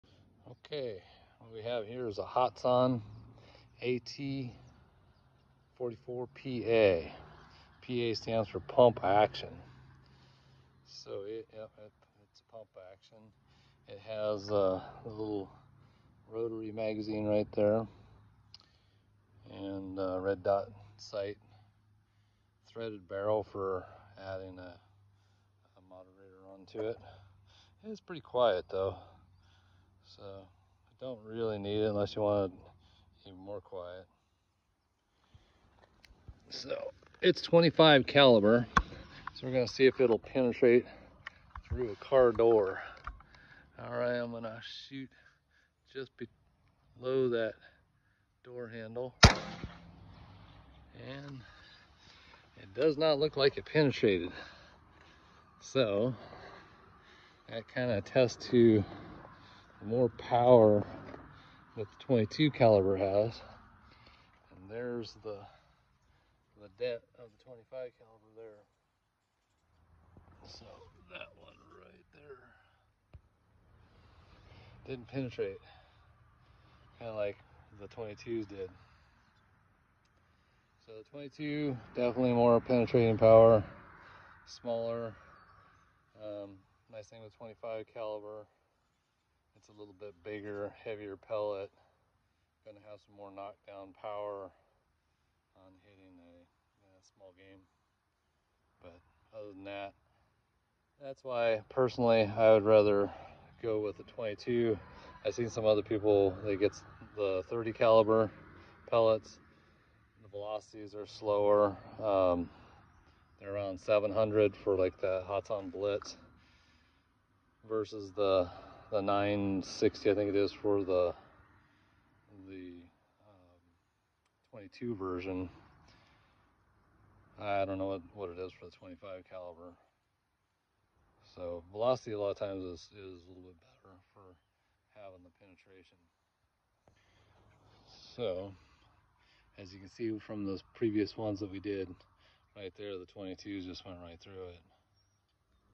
.25 cal pellet penetration test car door.
Hatsan AT 44-pa PCP pump action precharged pneumatic testing penetration on a car door.